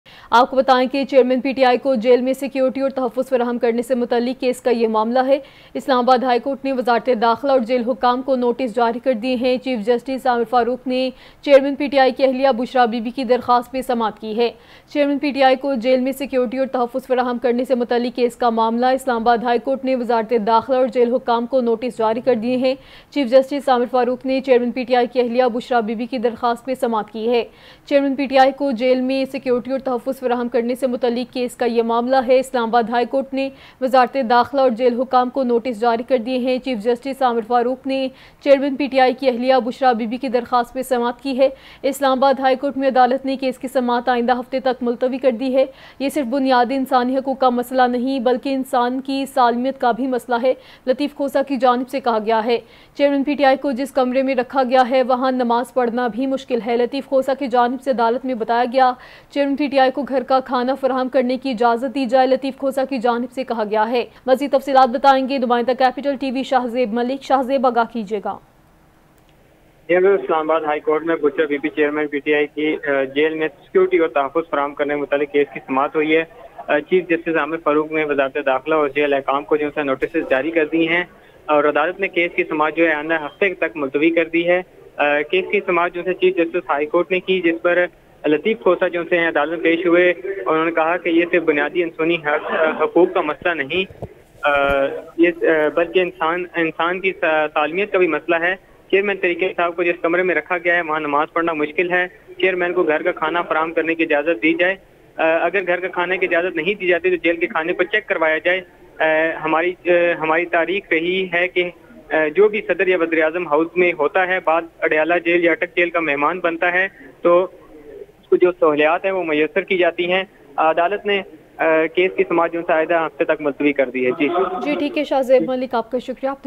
Latif Khosa Speak Out In front of Chief Justice Islamabad High Court | Big News | Capital TV
Latif Khosa Speak Out Infront of Chief Justice Islamabad High Court | Big News | Capital TV